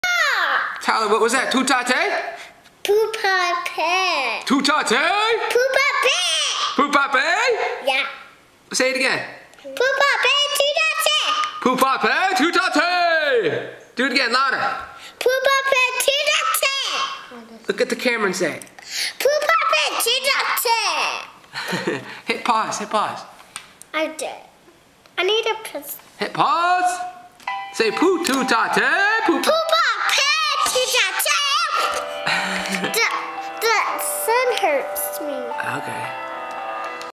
Pu pa pay Tu ta tay
Feb 2015 Tyler's saying, "Pu Pa Pay, Tu Ta Tay